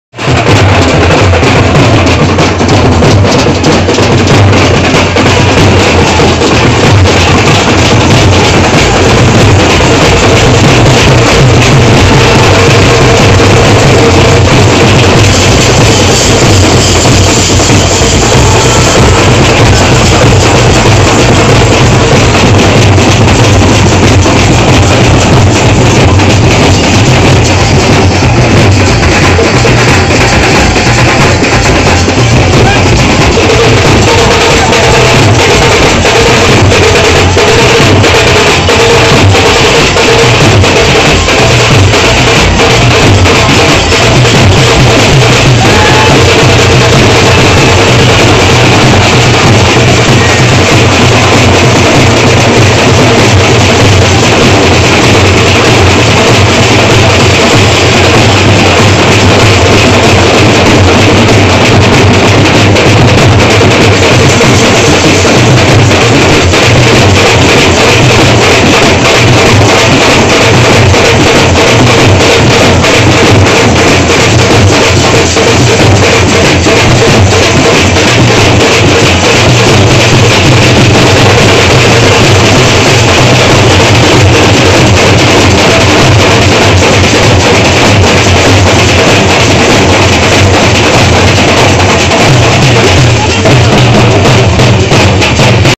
Happy Ramnavmi / Mishra Fans Club / 2022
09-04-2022 बाँधडीह जारीडीह प्रखण्ड मे राम नवमी का जलूस रात्रि 9 बजे निकालते हुए !